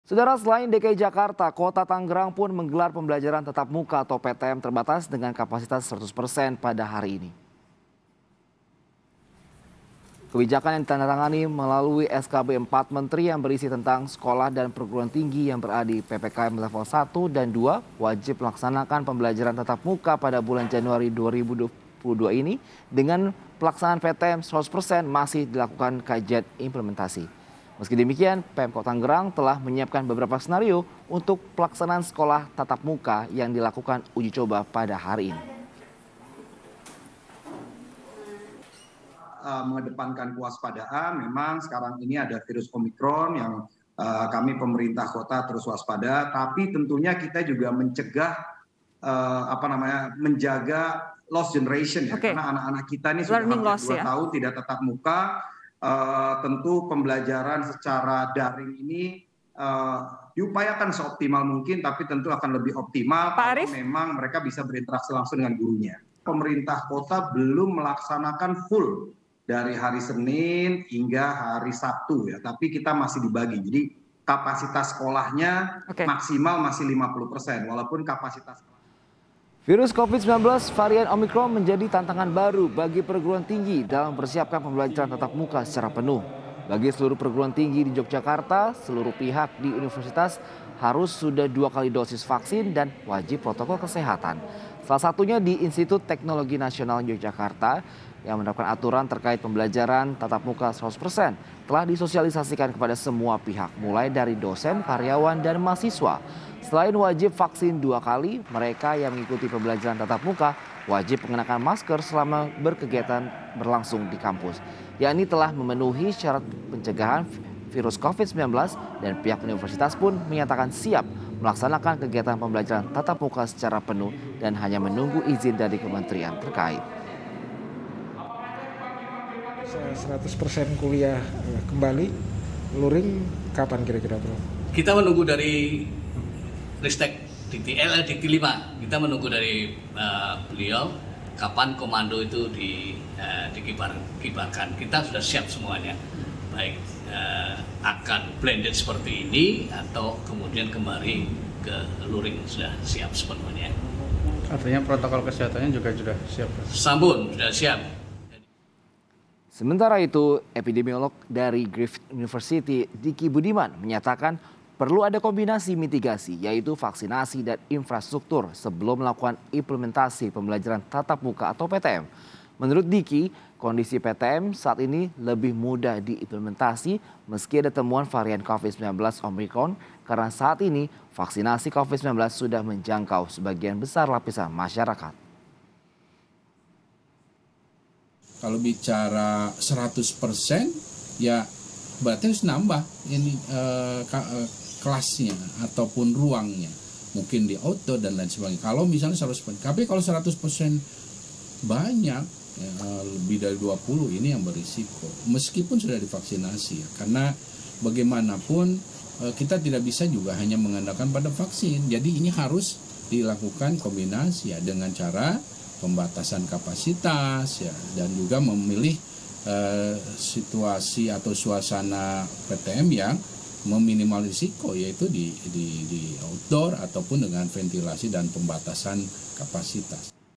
PTM Dimulai, Epidemiolog: Sekolah Tambah Kapasitas dan Bangun Infrastruktur yang Tidak Abai Covid-19
KOMPAS.TV - Epidemiolog dari Griffith University, Dicky Budiman menyatakan, perlu ada kombinasi mitigasi yaitu vaksinasi dan infrastruktur sebelum melakukan implementasi pembelajaran tatap muka (PTM).

Menurut Dicky, kondisi PTM saat ini lebih mudah diimplementasi meski ada temuan varian Covid-19 Omicron, karena saat ini vaksinasi Covid-19 sudah menjangkau sebagaian besar lapisan masyarakat.

Selain DKI Jakarta, Kota Tangerang pun menggelar PTM terbatas dengan kapasitas 100 persen, pada hari ini, Senin (3/1).

Baca Juga Uji Coba PTM Terbatas Berlaku 100 Persen di Jakarta, Apakah Ada Protokol Kesehatan Baru? di

Kebijakan yang ditandatangani melalui SKB 4 Menteri, yang berisi tentang sekolah dan perguruan tinggi yang berada di PPKM Level 1 dan 2, wajib melaksanakan pembelajaran tatap muka pada bulan Januari 2022 ini dengan pelaksanaan PTM 100 persen masih dilakukan kajian implementasi.

Meski demikian, Pemkot Tangerang telah menyiapkan beberapa skenario untuk pelaksanaan sekolah tatap muka yang dilakukan uji coba pada hari ini.